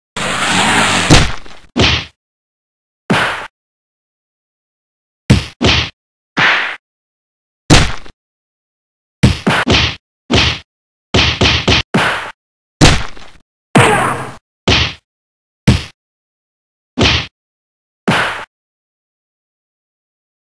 crazy stickman 2
crazy stickman 2 this time the blue stickman wants pay back. that means its not over the fight. hope you like it?